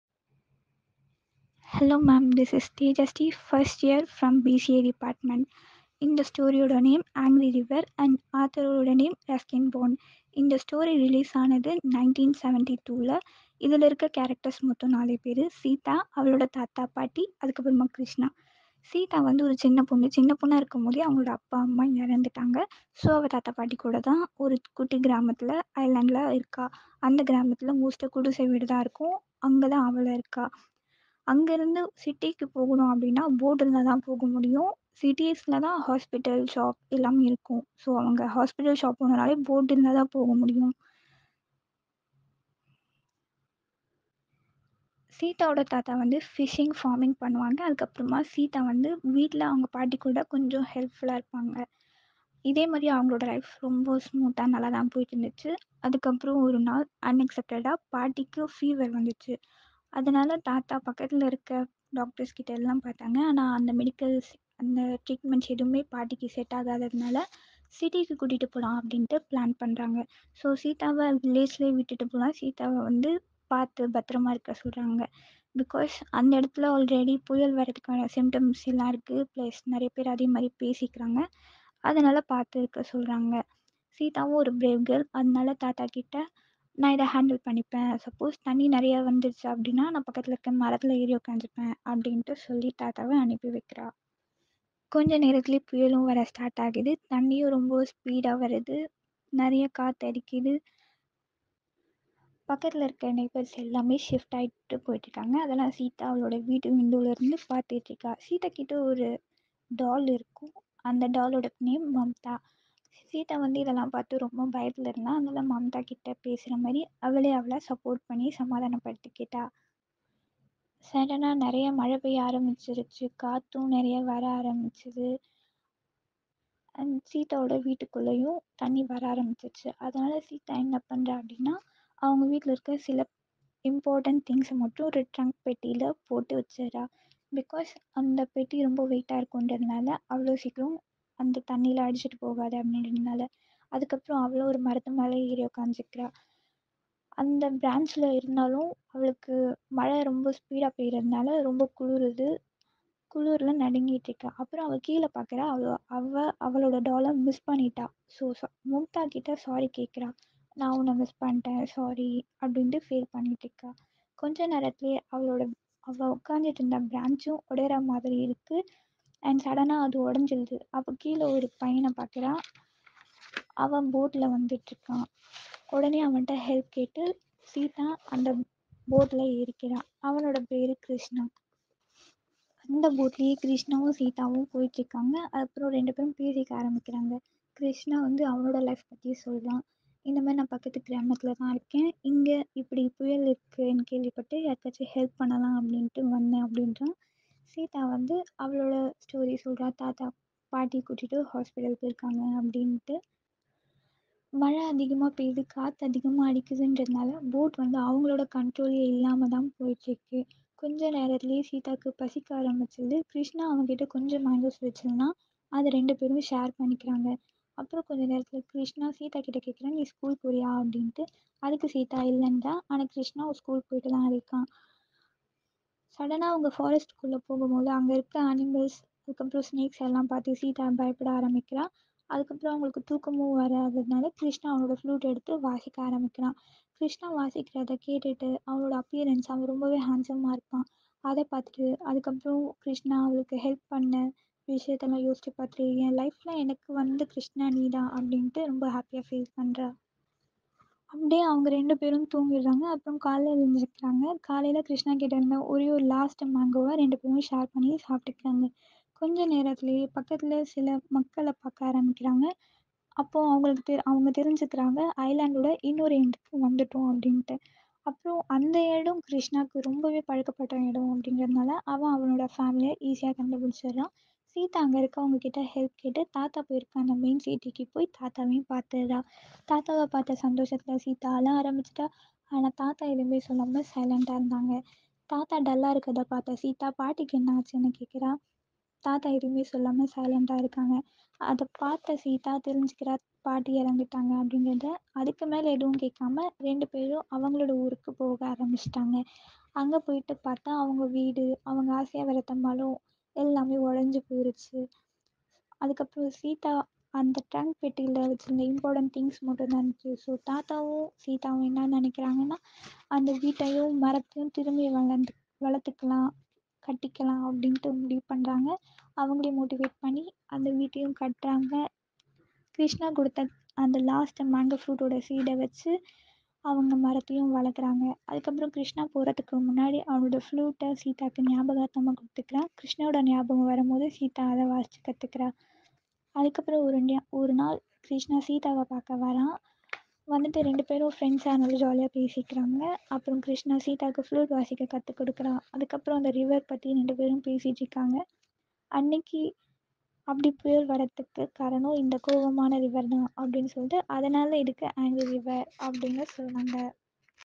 MWC Book a Day Book 463
Name :C.Theja Sree
BCA 1st year
Book name :The Angry river
Author: Ruskin Bond